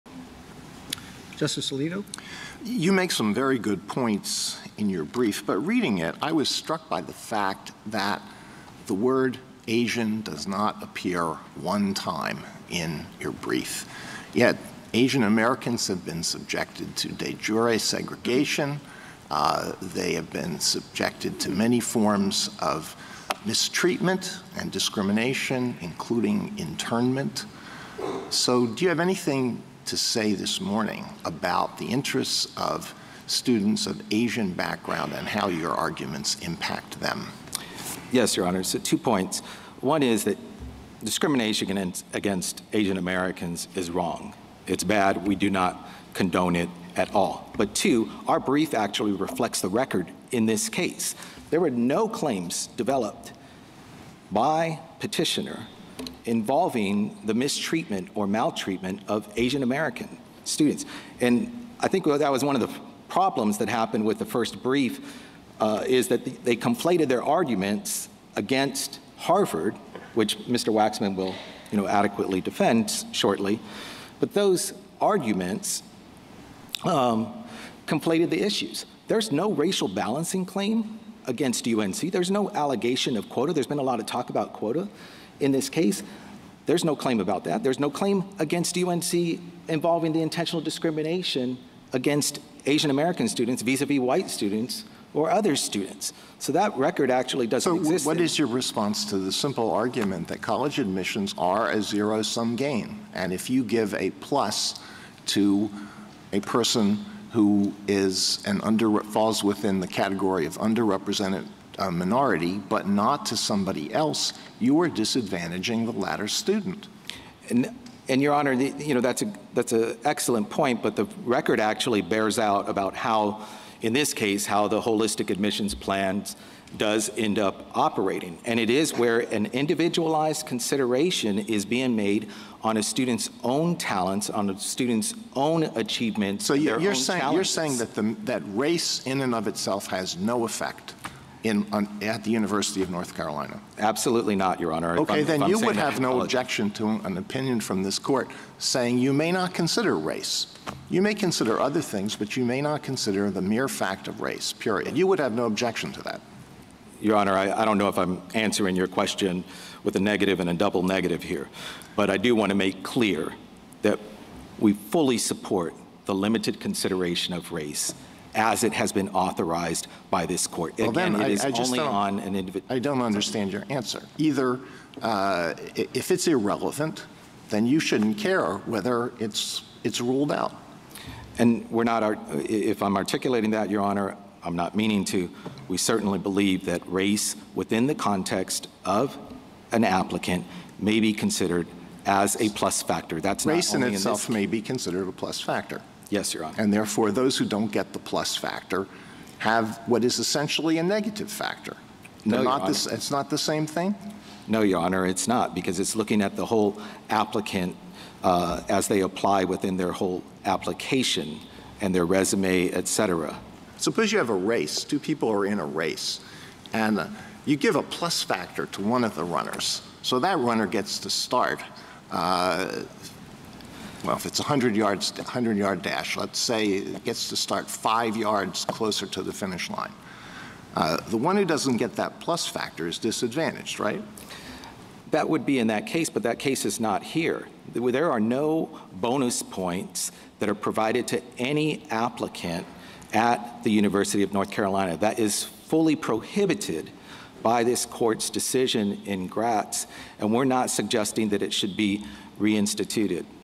‘The Word Asian Does Not Appear One Time In Your Brief’: Alito Grills Lawyer On Affirmative Action
On Monday, The Supreme Court heard oral arguments in Students for Fair Admissions v. University of North Carolina.